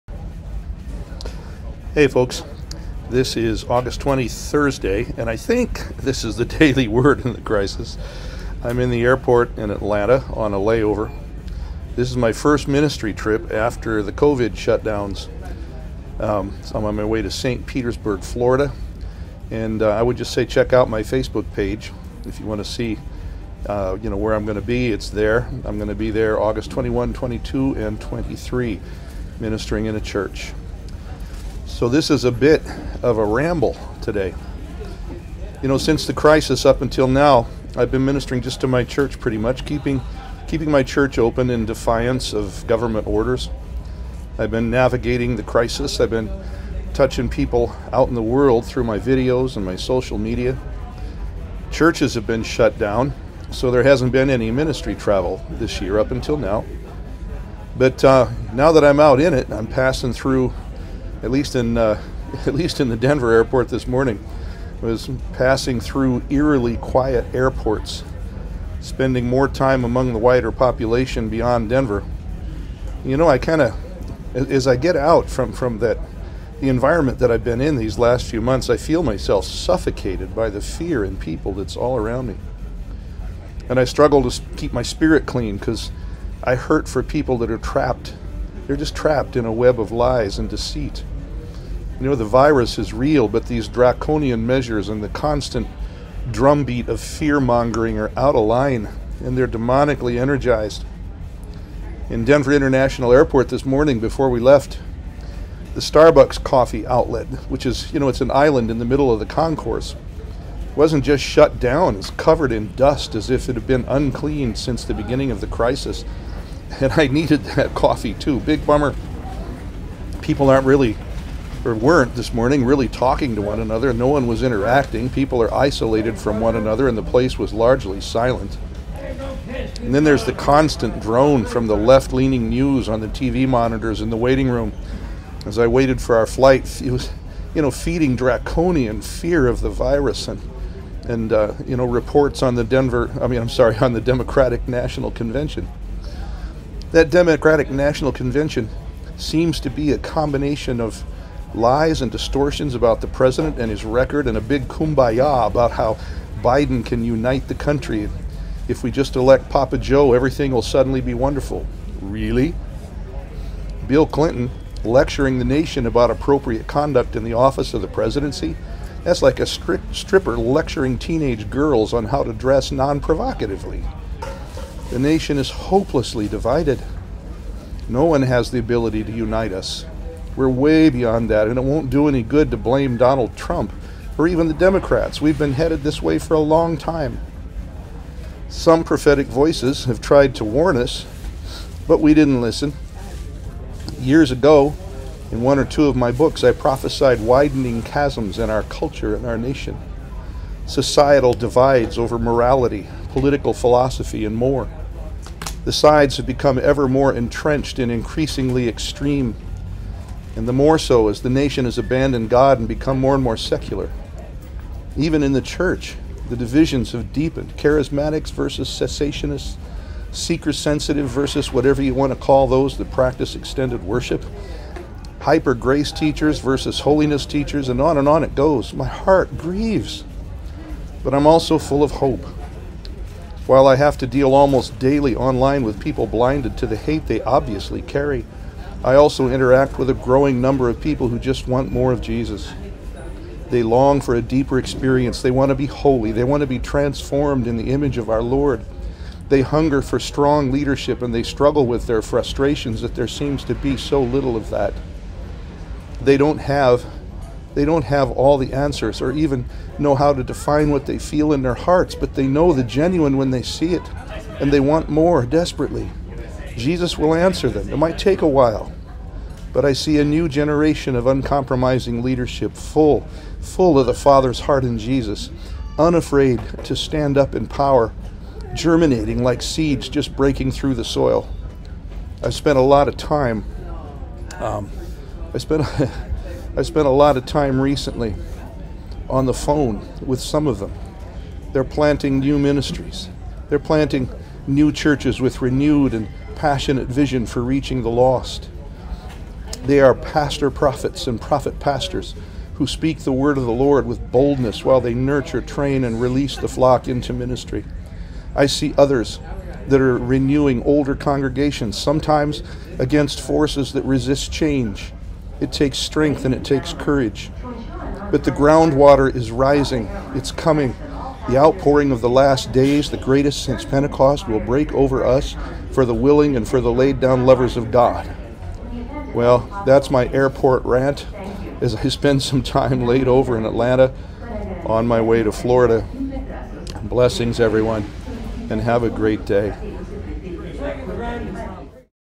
MY SENSE OF THE WORLD... - R. Loren Sandford with the Daily Word in the Crisis
From a Facebook Live broadcast.
New Song Church and Ministries
8242 Pecos St.
Denver, CO 80221
Senior Pastor: R Loren Sandford